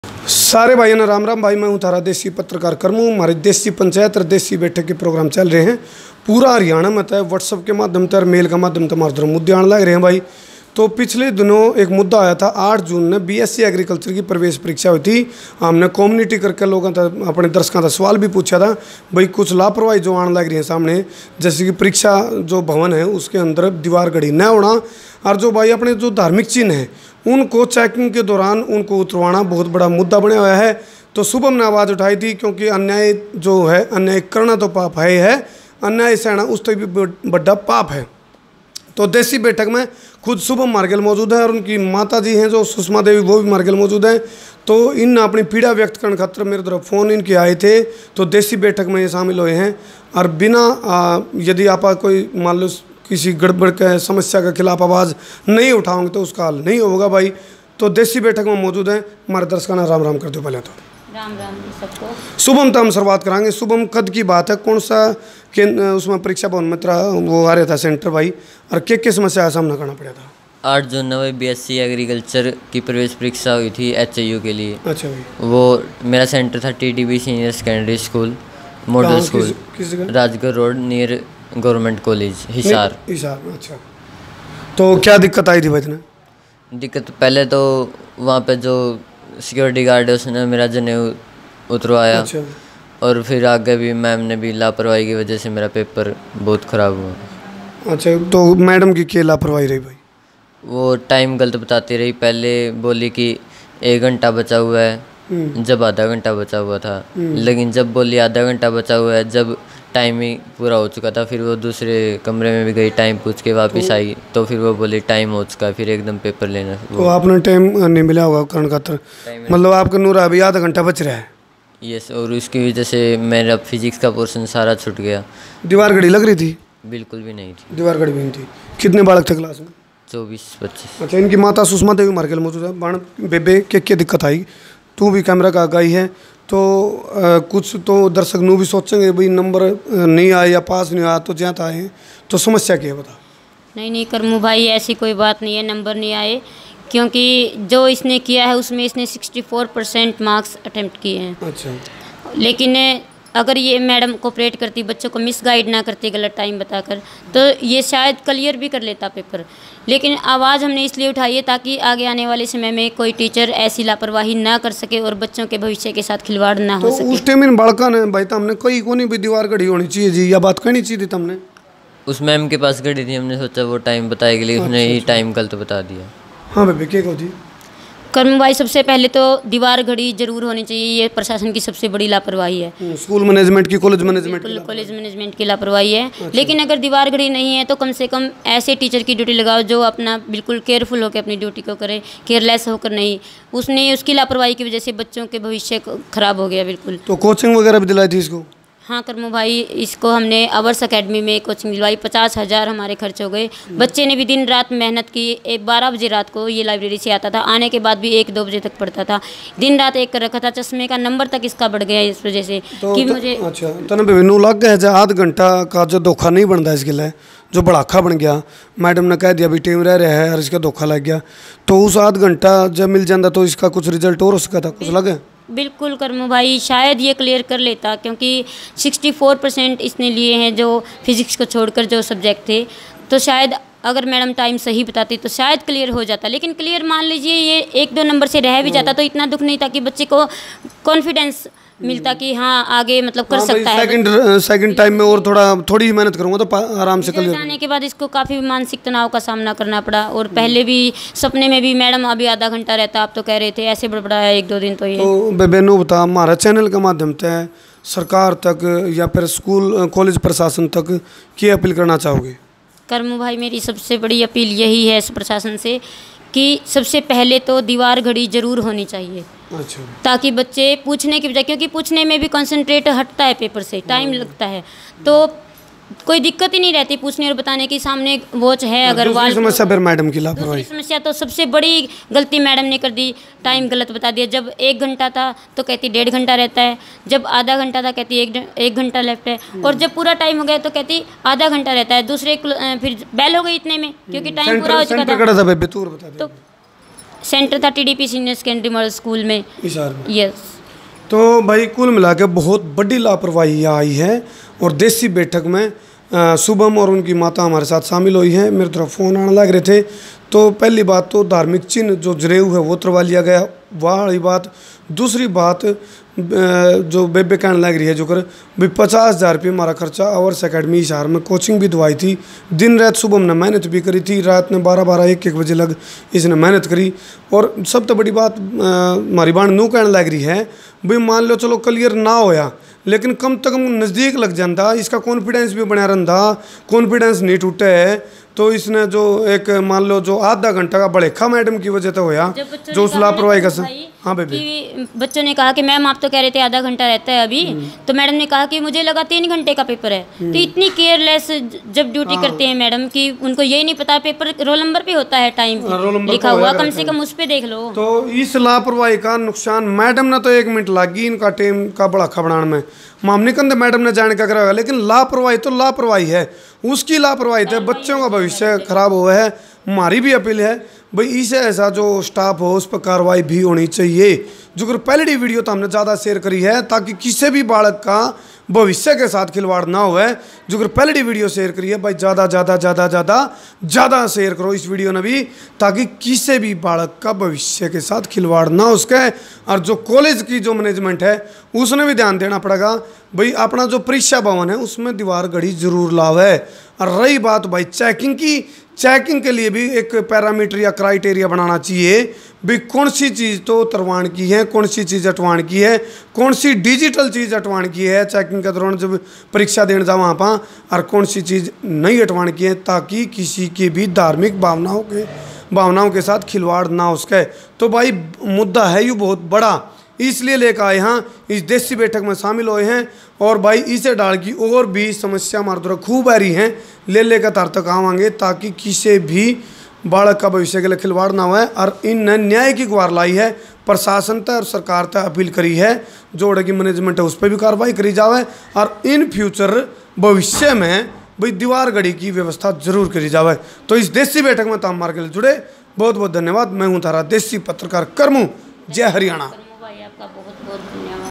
करमू की देसी बैठक में मां-बेटे ने लगाई न्याय की गुहार,BSC एग्रीकल्चर प्रवेश परीक्षा का मामला गहराया!
करमू की देसी बैठक में मां-बेटे ने लगाई न्याय की गुहार,BSC एग्रीकल्चर (HAU) प्रवेश परीक्षा का मामला गहराया! बड़ी लापरवाही के चलते बच्चे के भविष्य से हुआ खिलवाड़!